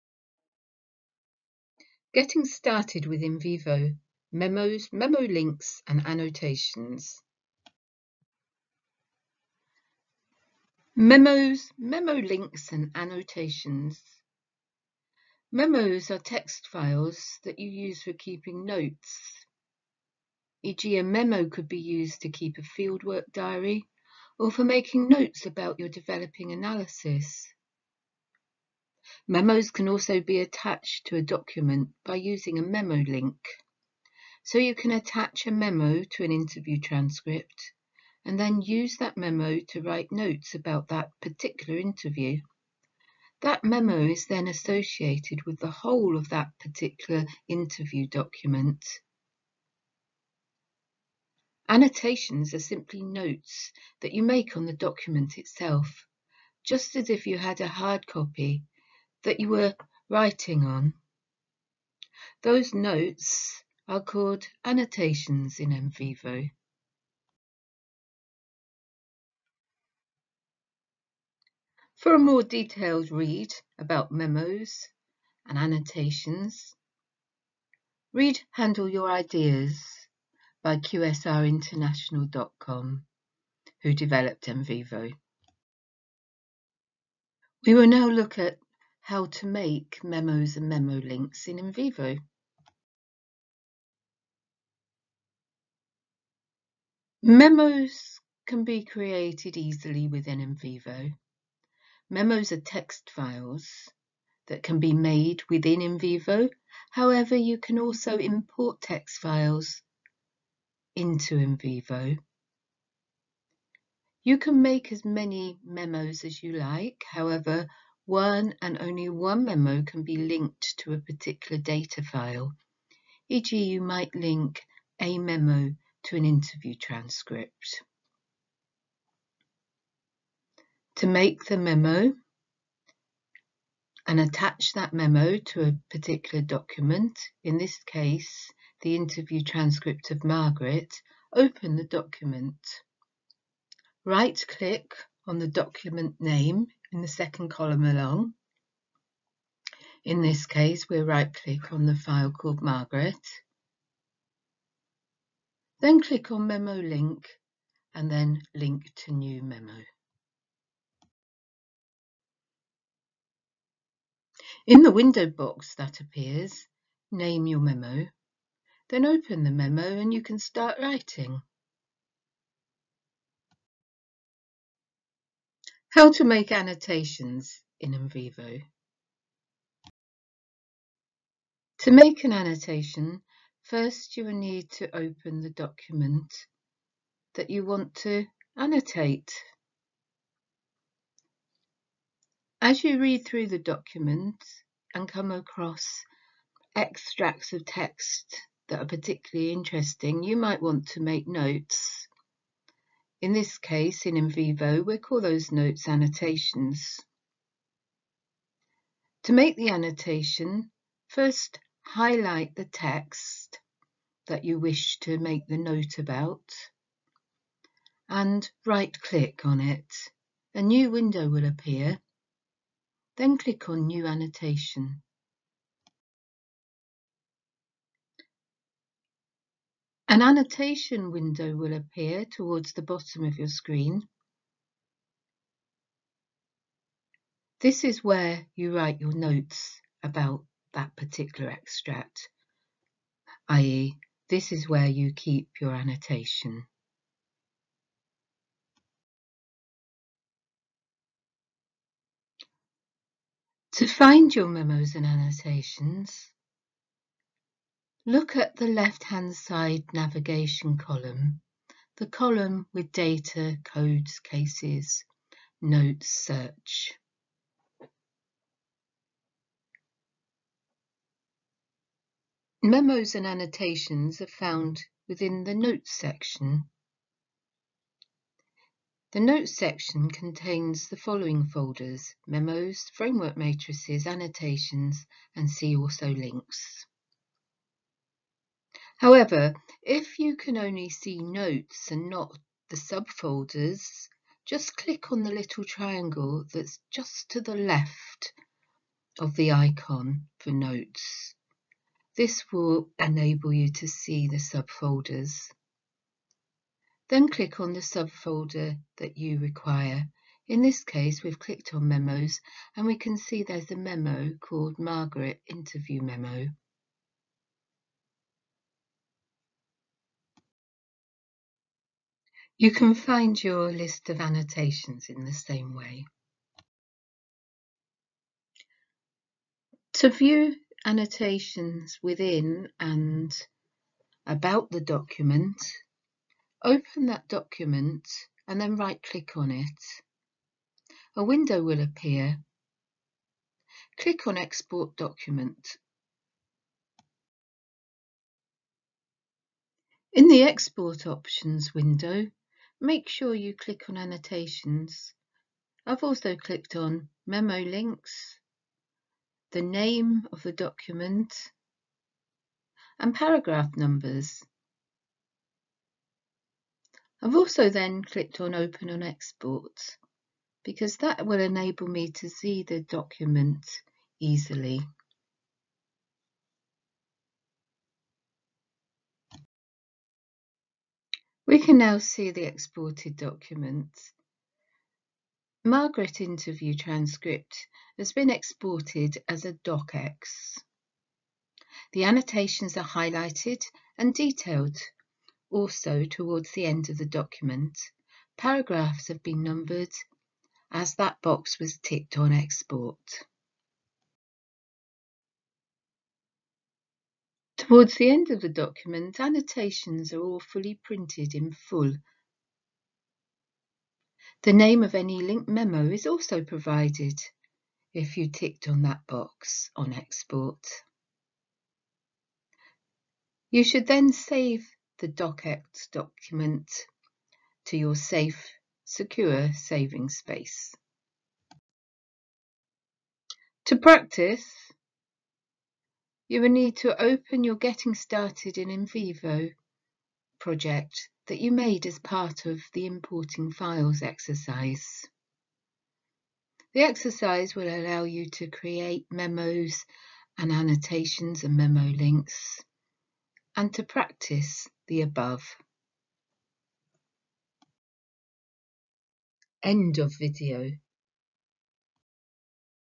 6. Memos, memo links and annotations in NVivo Windows
Introduction

0:00 - 0:02: Opening remarks
0:02 - 0:09: Overview of getting started with NVivo: memos, memo links, and annotations
Understanding Memos

0:09 - 0:25: Definition and purpose of memos
0:25 - 0:34: Attaching memos to documents using memo links
0:34 - 0:47: Association of memos with documents
Annotations Explained

0:47 - 1:12: Definition and purpose of annotations
1:12 - 1:23: Reference to "Handle Your Ideas" for more details
Creating Memos and Memo Links

1:23 - 1:58: Creating and importing memos in NVivo
1:58 - 2:16: Linking memos to specific documents
2:16 - 2:44: Step-by-step guide to linking a memo to a document
Making Annotations

2:44 - 3:50: Step-by-step guide to creating annotations
3:50 - 4:13: Writing notes in the annotation window
Finding Memos and Annotations

4:13 - 5:02: Navigating to the notes section and finding memos and annotations
5:02 - 5:22: Viewing memos and annotations
Exporting Documents

5:22 - 6:07: Exporting documents with annotations and memo links
6:07 - 6:21: Viewing the exported document
Practicing in NVivo

6:21 - 7:09: Instructions for practicing memos and annotations
7:09 - 7:28: Closing remarks and end of video